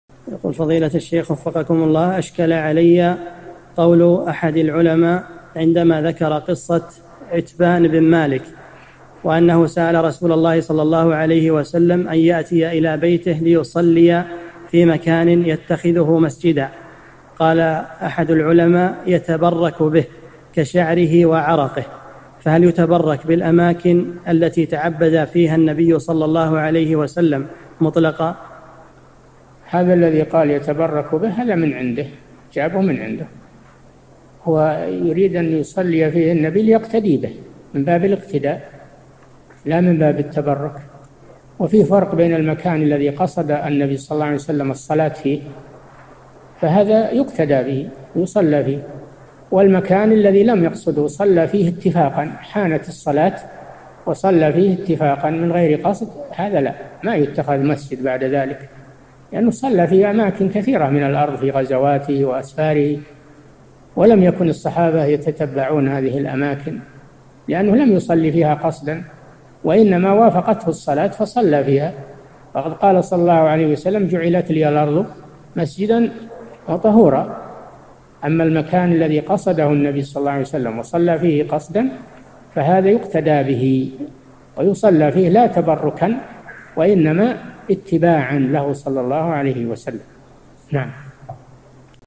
[668 /1350] سأل عتبان الرسولﷺ أن يأتي إلى بيته ليصلي في مكان يتخذه مسجدا قال أحد العلماء: يتبرك به!
-[668 /1350] السؤال: أشكل علي قول أحد العلماء عندما ذكر قصة عتبان بن مالكٍ، وأنه سأل رسول الله صلى الله عليه وسلم أن يأتي إلى بيته ليصلي في مكانٍ يتخذه مسجدًا. قال أحد العلماء: يتبرك به، كشعره وعرقه، فهل يتبرك بالأماكن التي تعبد فيها النبي صلى الله عليه وسلم مطلقًا؟
 الجواب: هذا الذي قال: يتبركُ به، هذا من عنده، جابه من عنده. هو يُريد أن يُصلي فيه النبي ليقتدي به، من بابِ الاقتداء، لا من باب التبرك، وفيه فرق بين المكان الذي قصدَ النبي صلى الله عليه وسلم الصلاة فيه؛ فهذا يُقتدى بهِ ويُصلى فيه، والمكان الذي لم يقصدهُ صلى فيهِ اتفاقاً، حانت الصلاة وصلى فيه اتفاقاً من غيرِ قصد، هذا لا، ما يُتخذ مسجد بعد ذلك؛ لأنه صلى في أماكن كثيرة من الأرض في غزواتهِ وأسفارهِ، ولم يكن الصحابة يتتبعون هذه الأماكن، لأنه لم يُصلِّ فيها قصداً، وإنما وافقتهُ الصلاة فصلى فيها، وقد قال صلى الله عليه وسلم : «جعلت ليَّ الأرضُ مسجداً وطهورا». أما المكان الذي قصدهُ النبي صلى الله عليه وسلم وصلى فيهِ قصداً، فهذا يُقتدى بهِ ويُصلى فيه، لا تبركاً، وإنما اتباعاً له صلى الله عليه وسلم .
للاشتراك في قناة الشيخ صالح الفوزان
أسئلة وأجوبة من شرح قاعدة جليلة ( 1350 ) مقطع

للاستفسار والإقتراحات والدعم
رئيس الفريق
لمشروع كبار العلماء